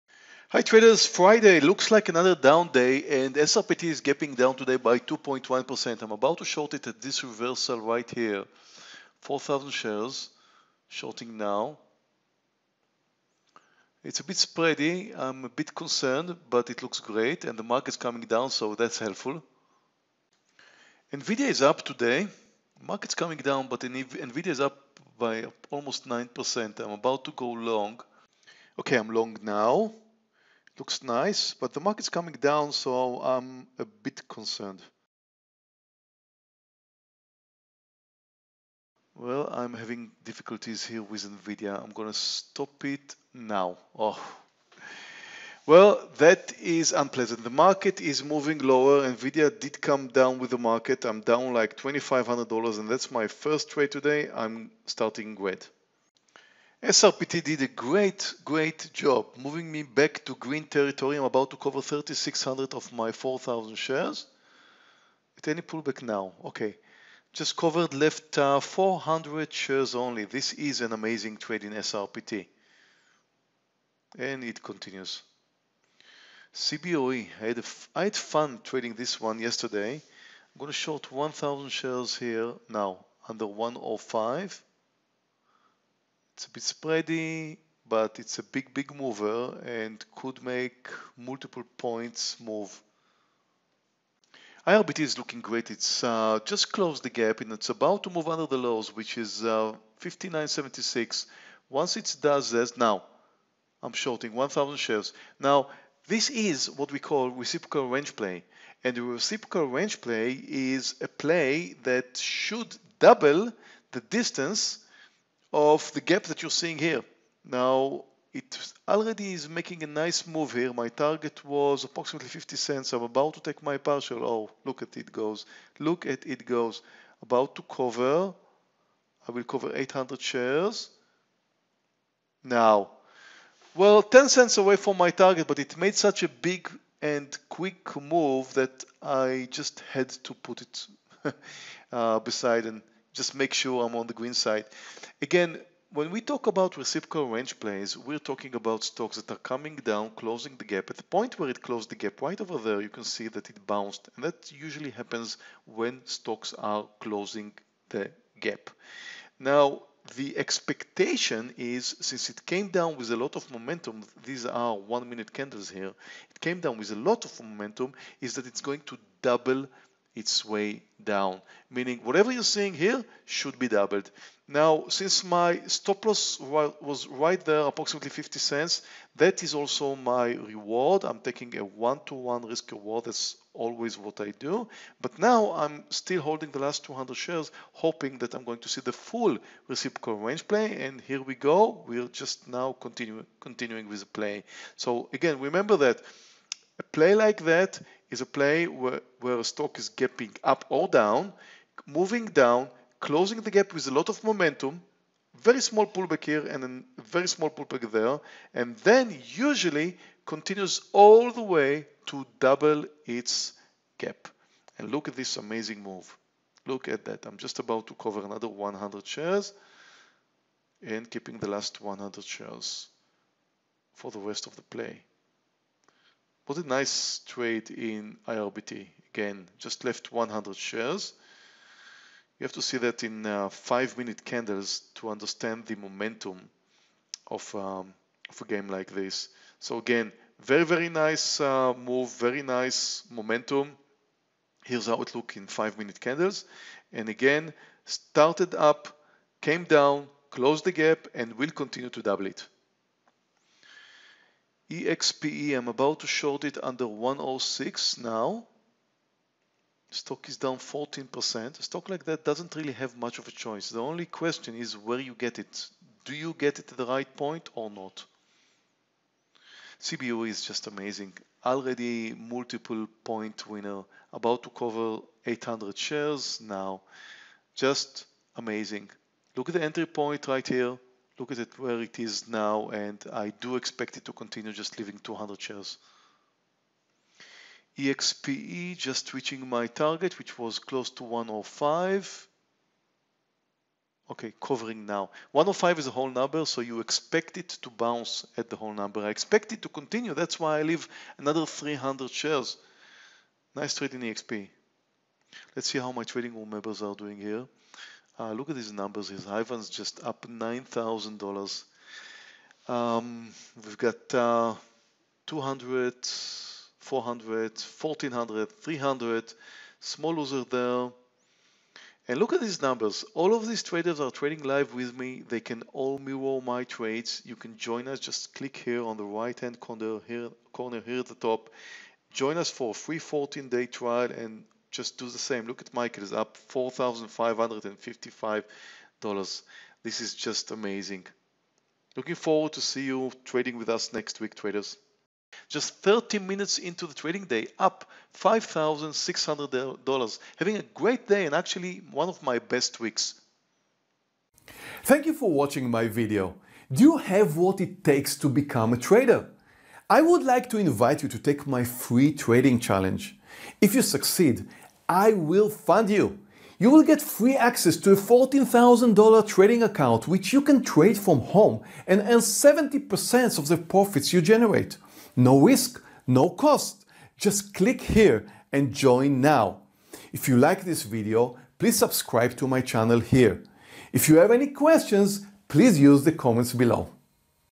Live Day Trading for $5,600 in 30 Minutes
Live Day Trading Stocks - How to make money by day trading stocks -  $5,600 in 30 minutes. in this video you can also learn about the reciprocal range play. Meir Barak day trading live on February 9th. 2018.